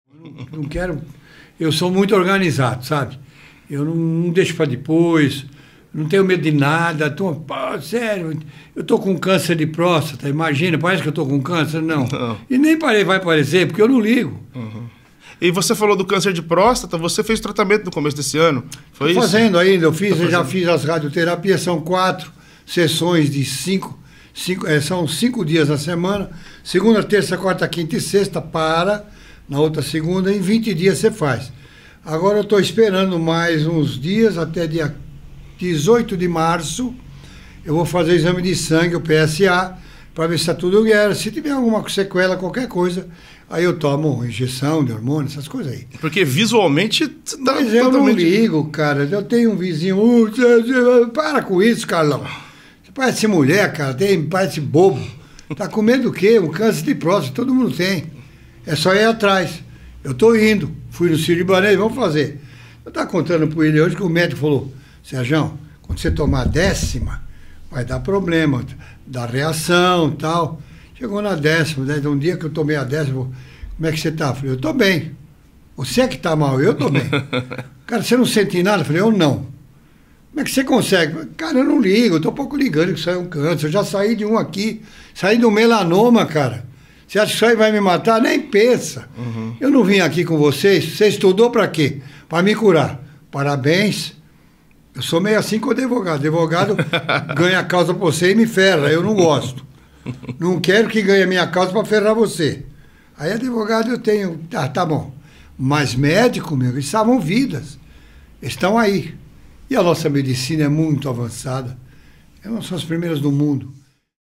COMO ESTÁ SENDO O TRATAMENTO DE SAÚDE DELE? | Piunti entrevista Sérgio Reis
Meu insta: @piunti.

Sérgio Reis é um dos nomes mais importantes da história da música sertaneja.

Aos 81 anos, segue firme sendo um exímio contador de histórias. Com opiniões firmes e muitas vezes polêmicas, foi assunto nos últimos anos por questões de política. Serjão foi, inclusive, deputado federal.

Durante a entrevista, falamos de questões políticas, mas o papo foi focado mesmo em música e história.

Sérgio Reis também fala de sua situação de saúde.

Quase duas horas de papo. Praticamente todos os hits do Serjão foram lembrados durante a conversa.

Um carnaval diferente em 2022.